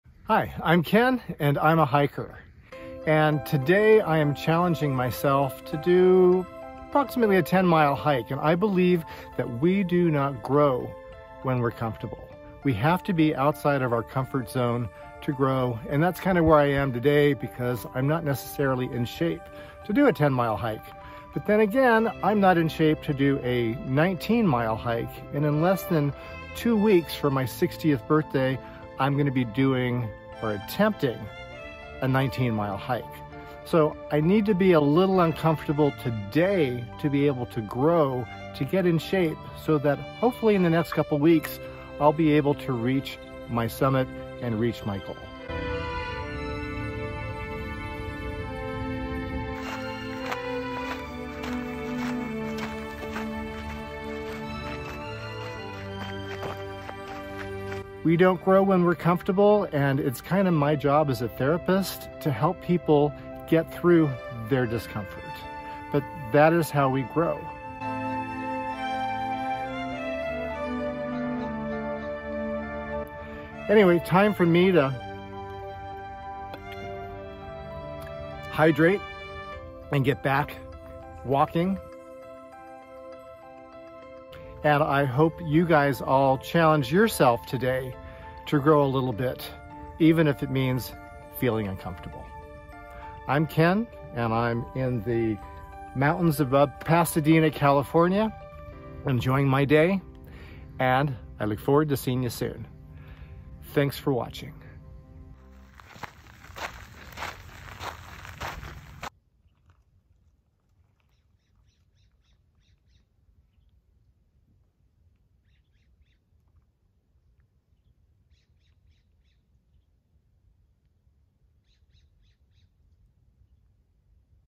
We Grow When We Are Uncomfortable
Feeling stuck in your comfort zone, yet unhappy with how your life is going?  Learn it's OK to be somewhat uncomfortable.  That is how we grow!

The Loving Parent Guidebook describes the difference between comfort, discomfort (our challenge zone) and distress (feeling overwhelmed).  Fear often keeps us comfortable and unchallenged.

I'm Ken Francis, MS, California licensed Marriage and Family Therapist and author of The Intimacy Gram and The Gratitude Snorkel.  With Covid-19 social distancing practices, I have had to stop teaching classes at my clinic.  This video and others are my attempt to make my educational topics available to my patients. I have placed them on YouTube so others can benefit.